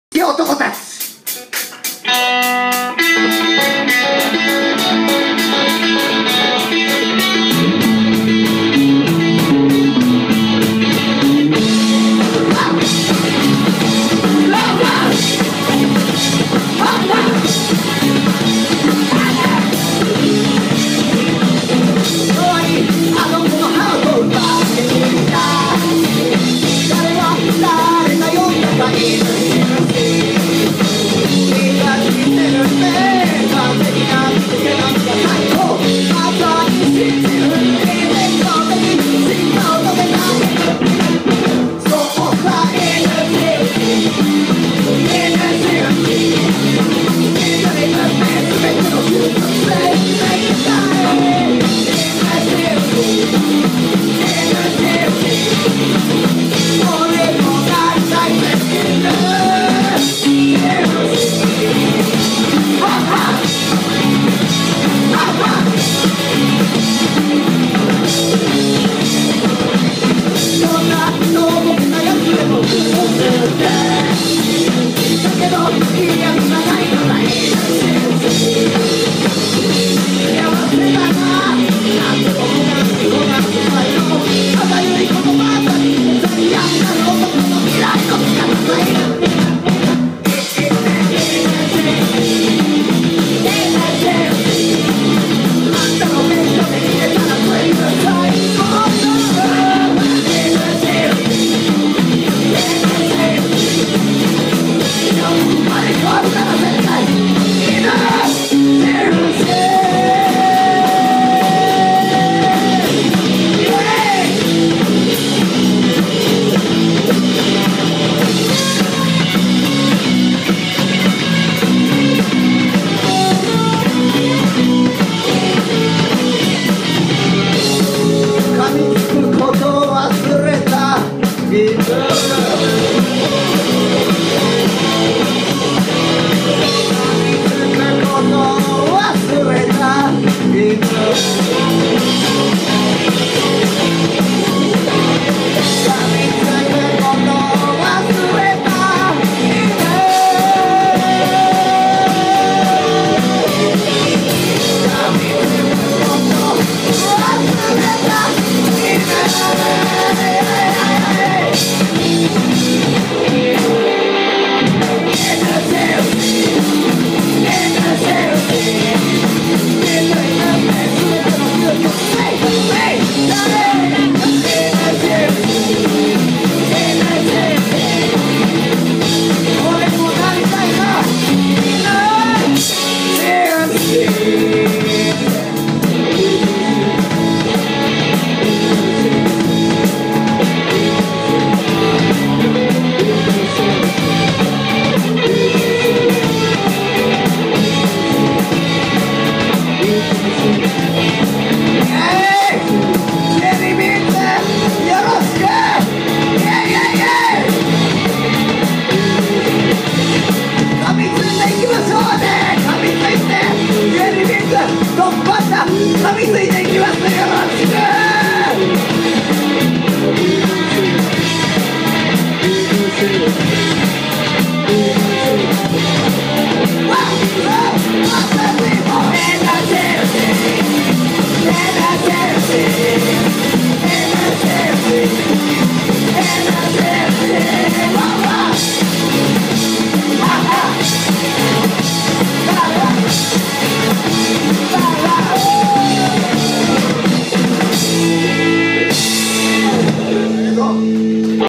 ジュエリービーンズ 2013年5月5日@難波Mele
2013年5月5日 日曜日
難波Mele 「Wooly  Bully！」でのライブ。